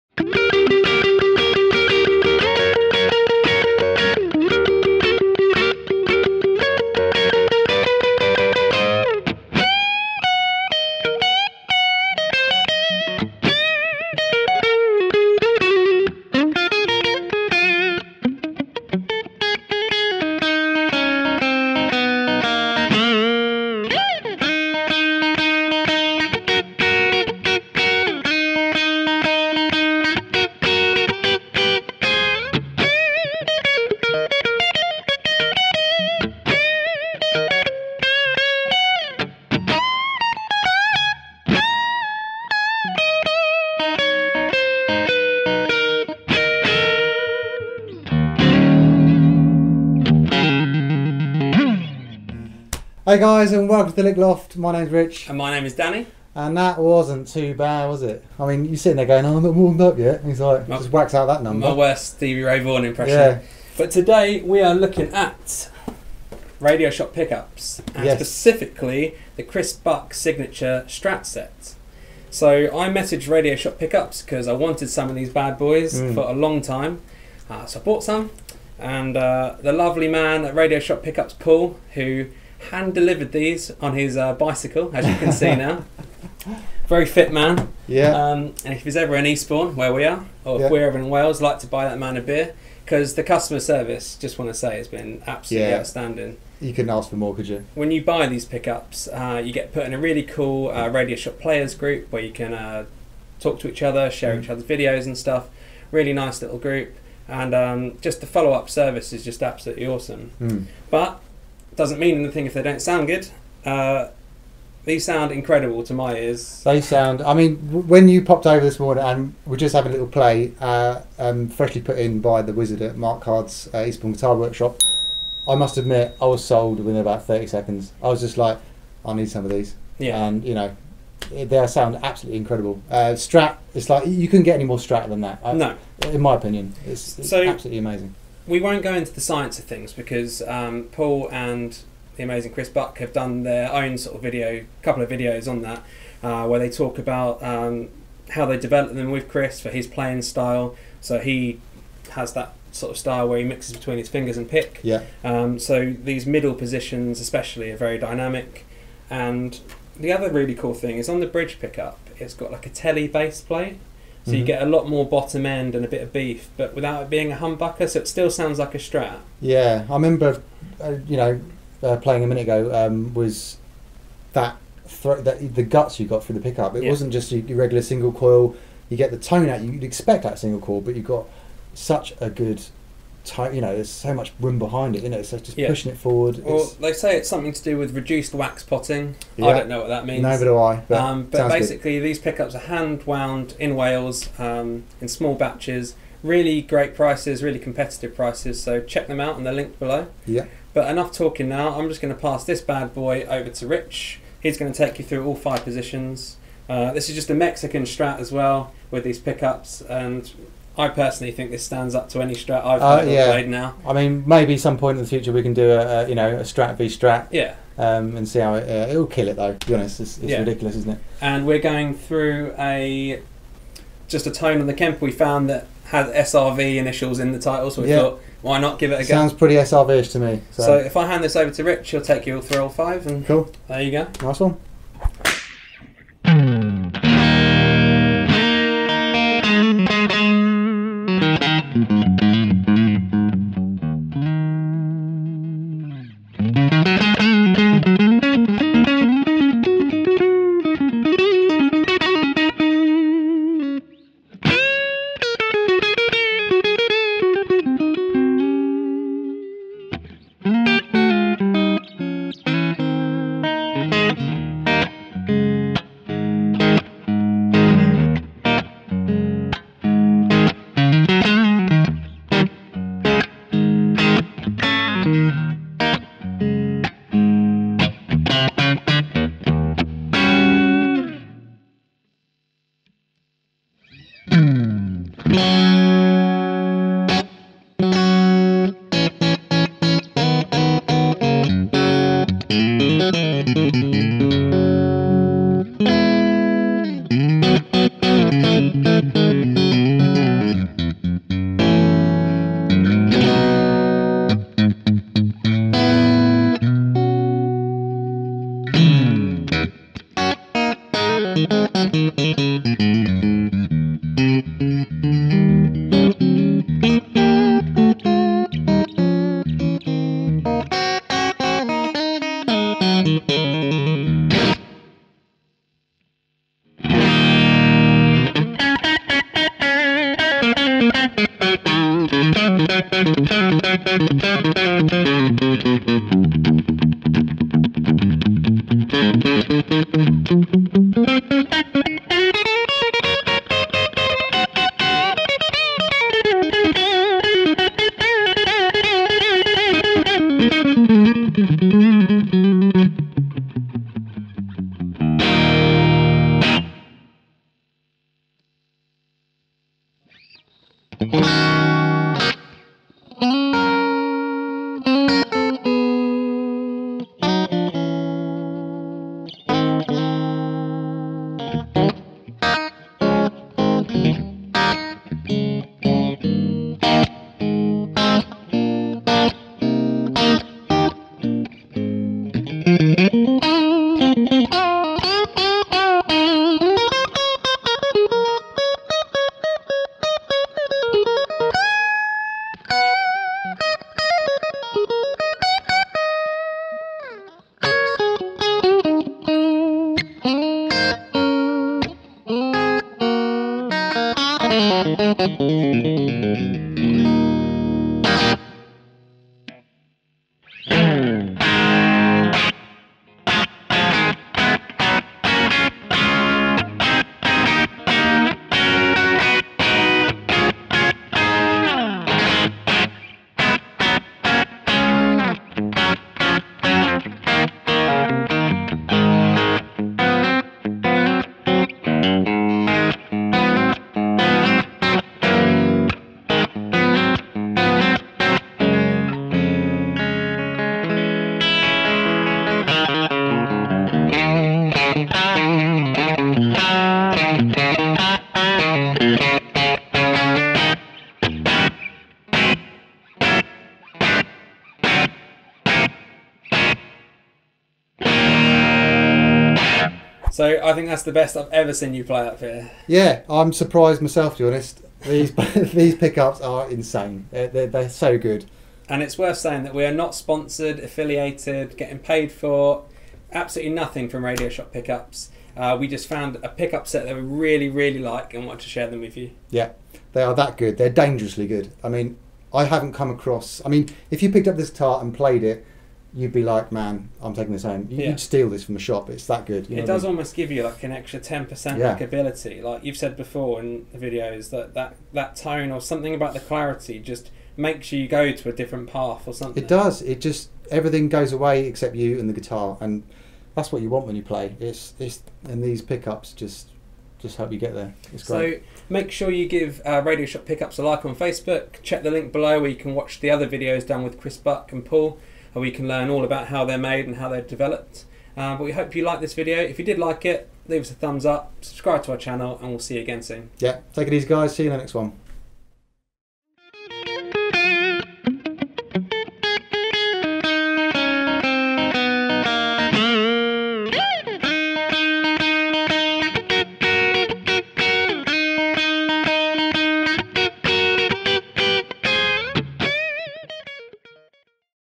Chris Buck Pickups | Radioshop Pickups Review
All pickup sounds at 3:55

In today's video we have a look at our favourite stratocaster pickups on the market today. They are made by Radioshop Pickups, based in Wales (UK). Are these the best strat pickups you have heard lately?

These are the ID: Chris Buck Signature Strat Pickup Set. The ID stands for increased dynamics. Chris Buck plays with a plectrum and his fingers, so every nuance is captured in these Chris Buck pickups. The in-between (positions 2-4) are so usable and dynamic, unlike some other stratocaster pickups we've tried before. The neck is the ultimate neck "stratty" sound and the bridge pickup has a "tele base plate" on the bottom which gives it some real bottom end and growl without losing the single coil sound or turning it into a humbucker.

All of their guitar pickups are handcrafted and hand wound in their workshop in Wales.

This isn't a sponsored video either, we just love these pickups and want to share them with you!

All work carried out to Danny's Fender strat was by The Eastbourne Guitar Workshop

We used the Kemper Profiling Amp for this video. We tend to just look through the Kemper Rig Packs for a sound we like but if you'd like to know what profiles we use, comment below and we'll let you know.

Are these the best stratocaster pickups you have heard lately? Let us know below what you thought! We value and reply to every comment!

Come and be our friends too!